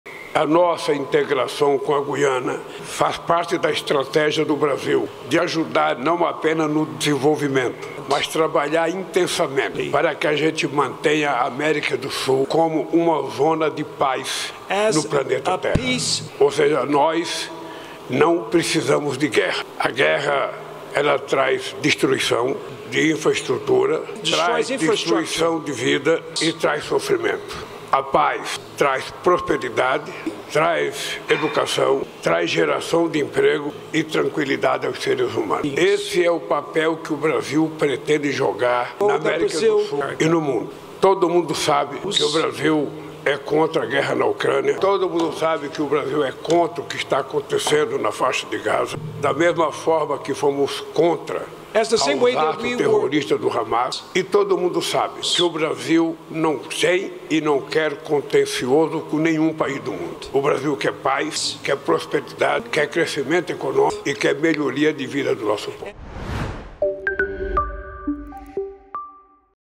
BRASIL QUER PAZ, PROSPERIDADE E MELHORIA DE VIDA DO POVO
A integração com a Guiana é parte da estratégia do Brasil, visando não apenas o desenvolvimento, mas também trabalhar incansavelmente para manter a América do Sul como uma zona de paz.
🎥 Audiovisual/PR